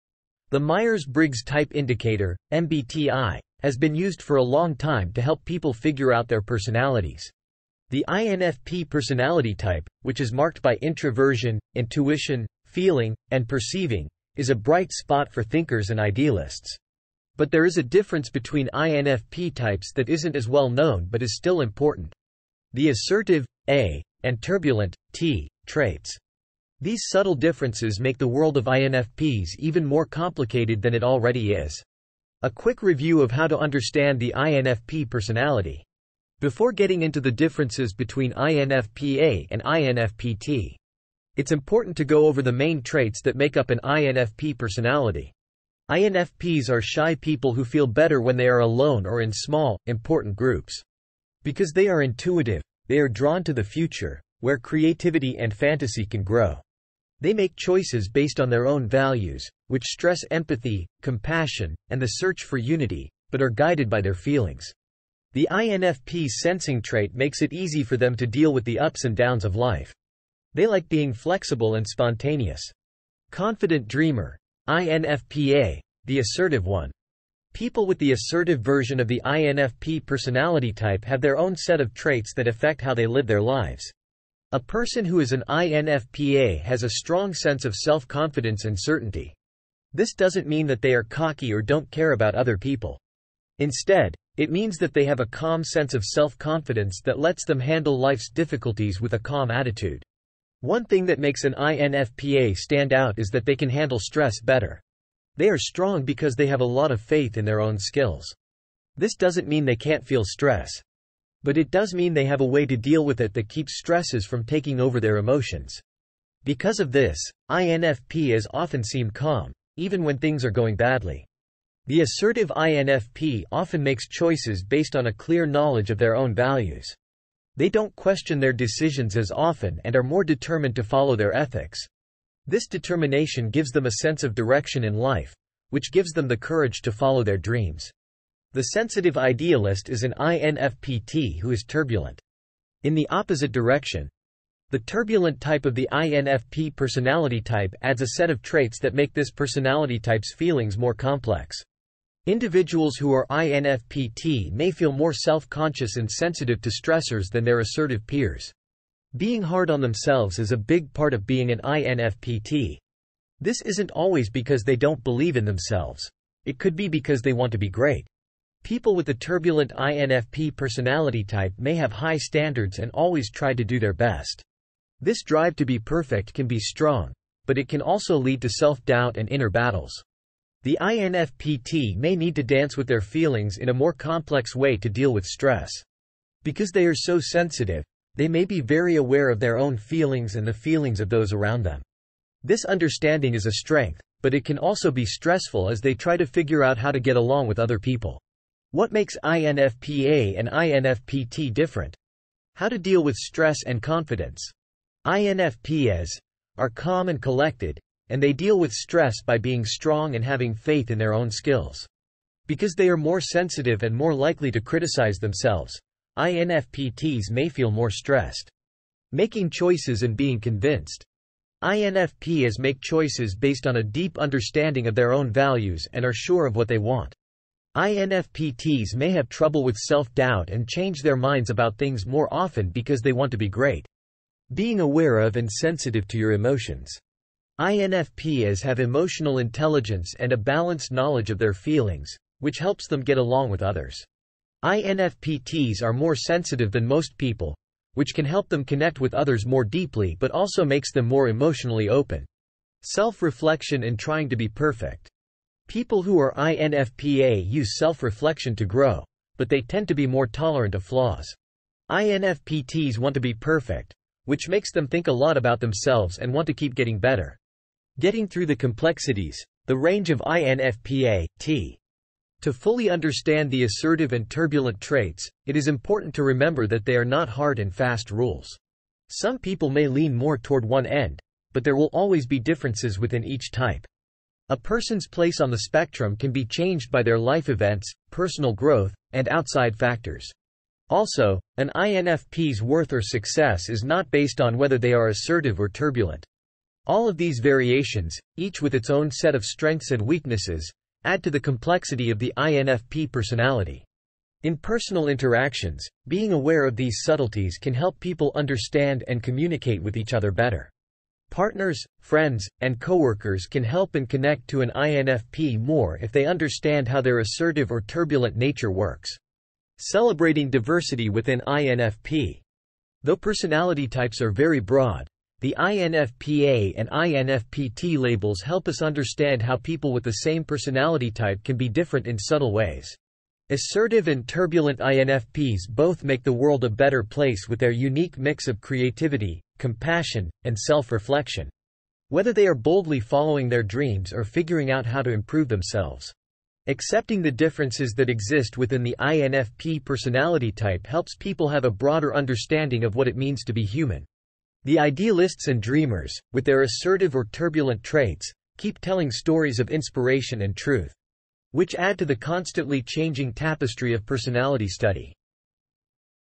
INFP-A VS INFP-T : Discover the Ultimate Battle of Personalities!
Dive into the fascinating world of personality types as we explore the clash between INFP-A and INFP-T!

Uncover the nuances and differences in this ultimate showdown of personalities. Which type resonates with you? Join us on this journey of self-discovery and find out! INFP PersonalityTypes INFPvsINFP"